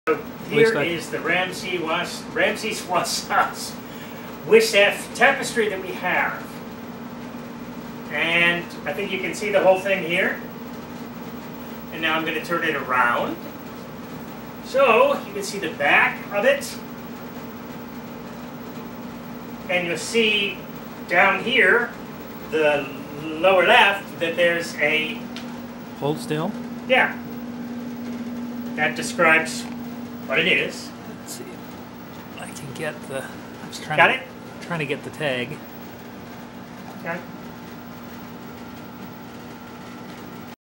Wissa Wassef US
Wissa Wassef tapestry available for sale in the Unitest States by artist Fatma Awad under the direction of Sophie Habib Gorgi, Chicago Illinois.